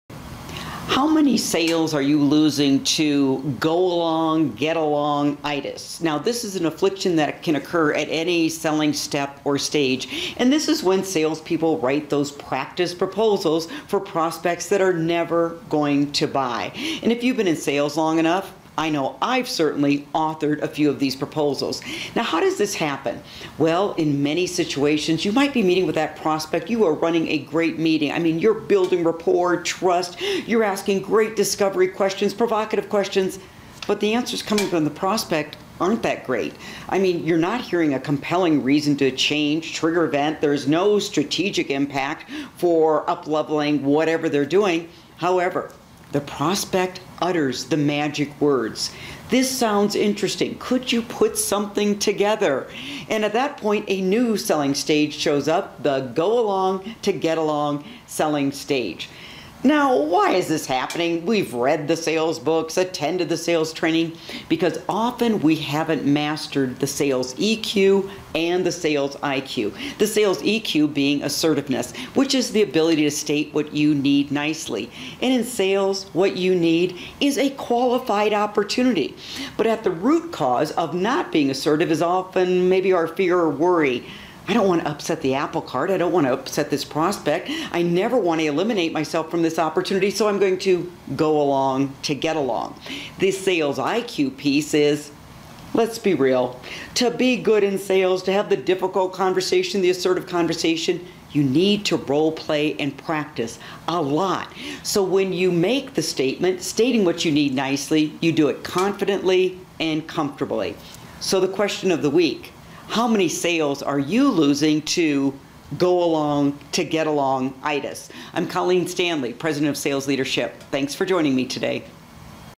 Are Your Sales Suffering From Go-Along-to-Get-Along-itis? | SalesLeadership, Inc.
Denver, Colorado sales consultant Colleen Stanley with SalesLeadership, Inc.,  discusses missing sales forecasts due to the lack of these two emotional intelligence skills.

Through sales training, seminars, workshops, and other services, SalesLeadership, Inc. helps to instill best in class sales strategies and tactics.

SalesLeadership, Inc.